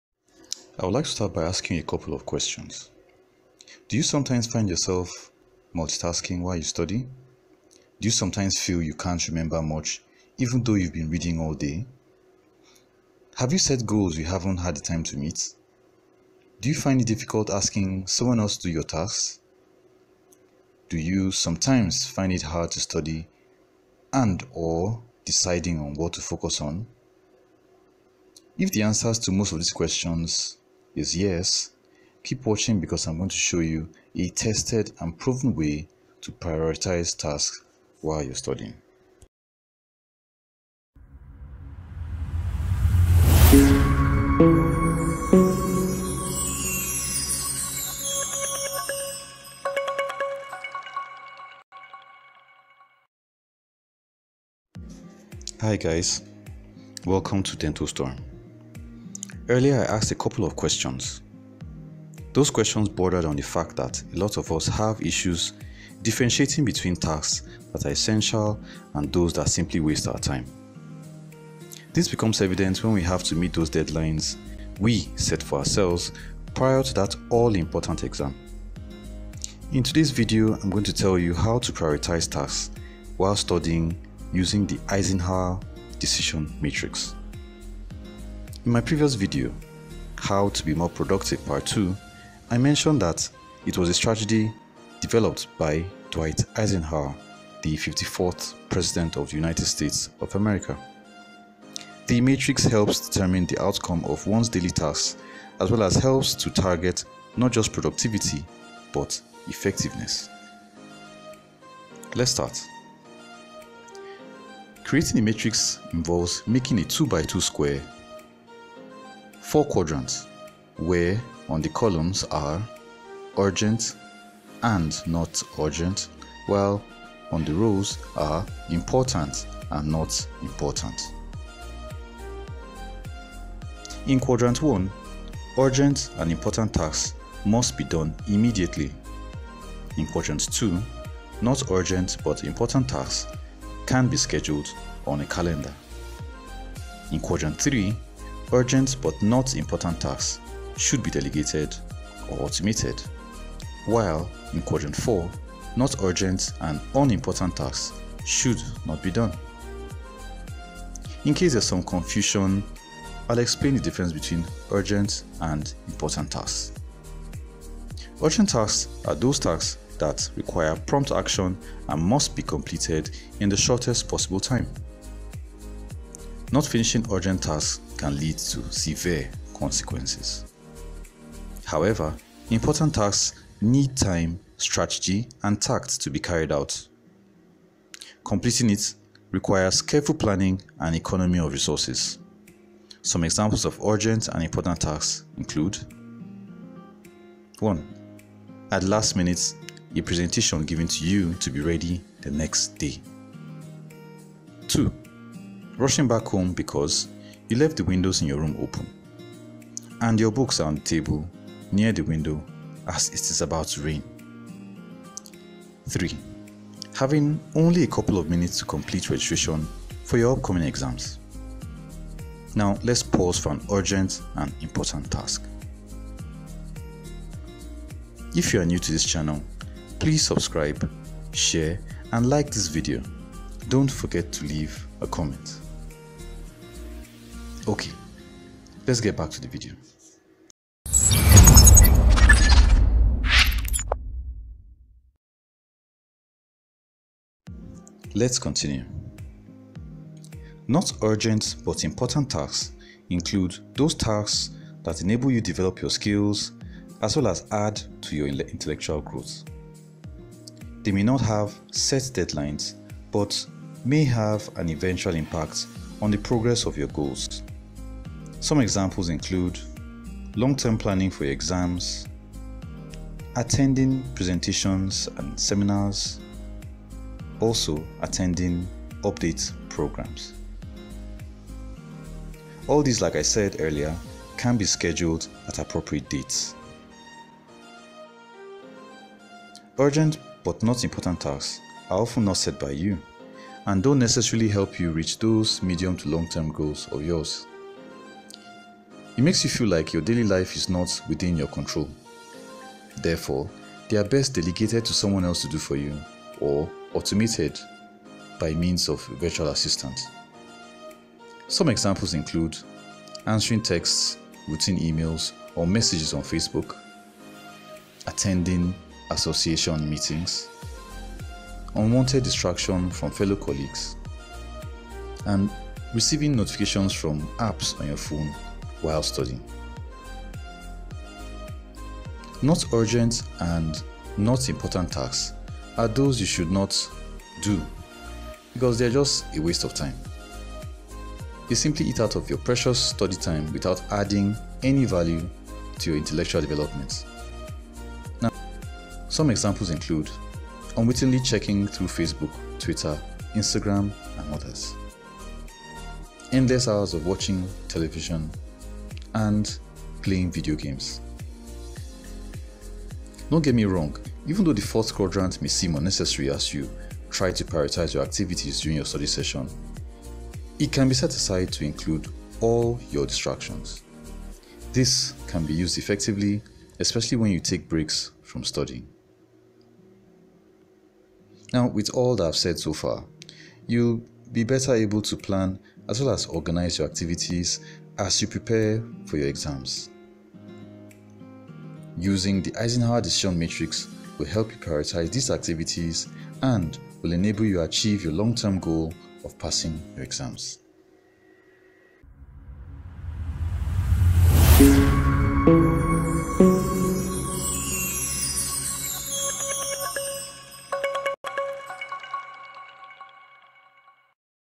The Eisenhower Decision Matrix ::: Prioritizing tasks
If you are in a dilemma as to knowing which tasks are important and which are useless why not try the Eisenhower Decision Matrix. This video will sure teach you how to prioritize tasks to get you settled to work or study well. Have fun watching